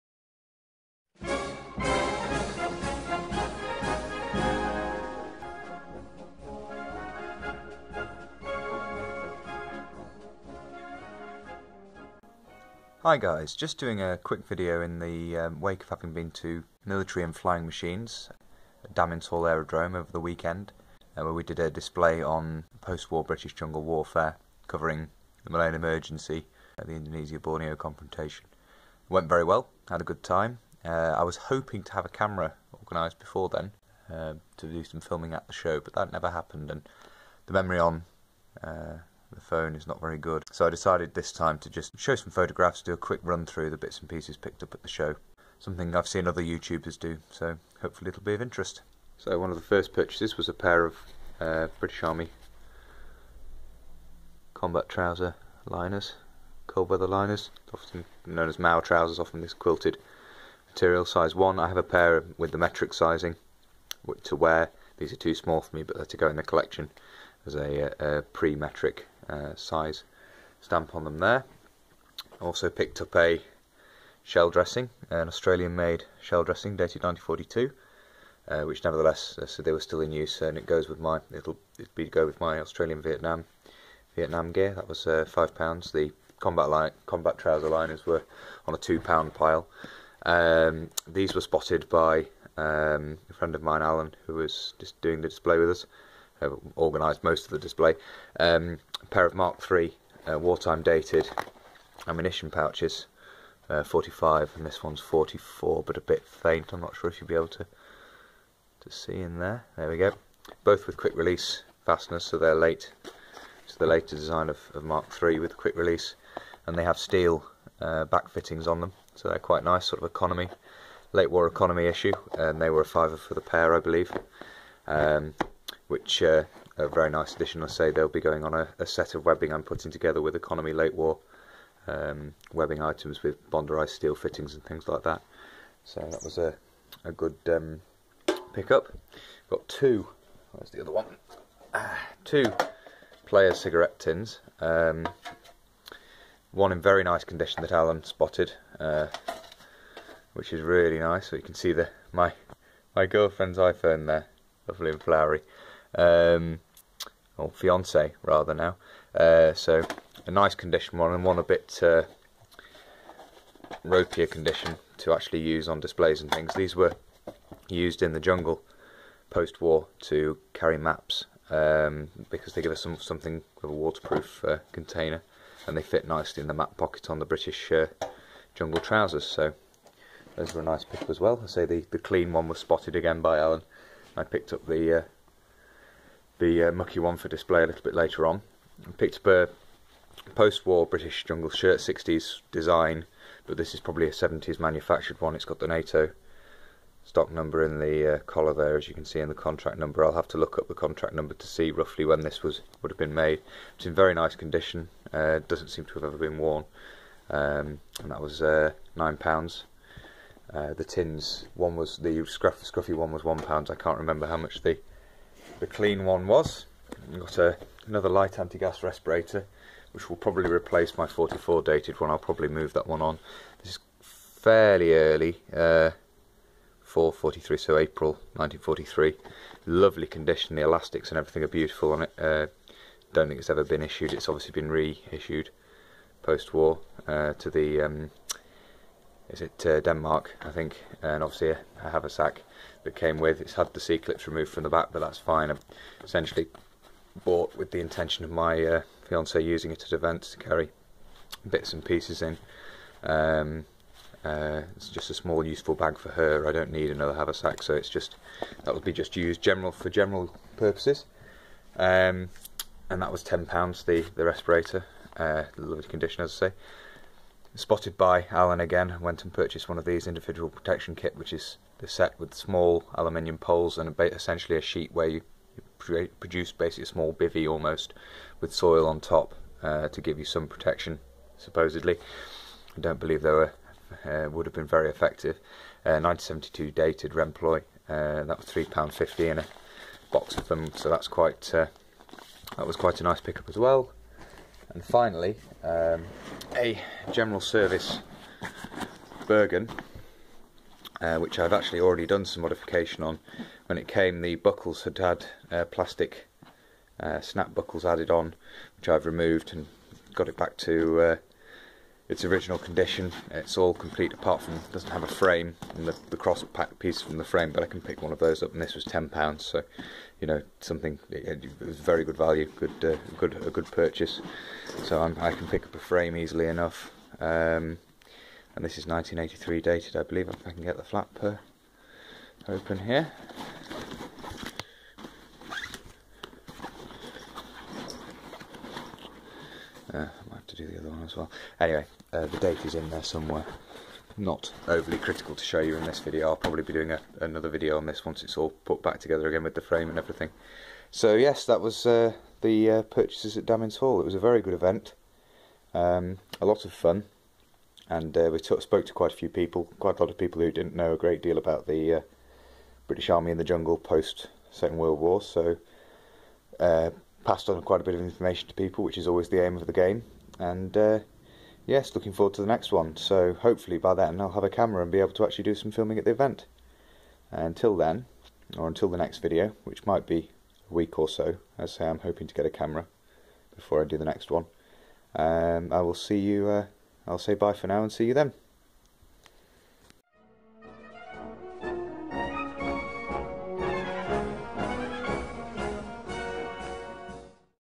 Military and Flying Machines, Damyns Hall 2016 - Show Report
A video showing a few stills from a recent display at Damyns Hall aerodrome covering the Malayan Emergency and the Indonesian Confrontation and the collection of bits and pieces picked up from stalls at the show.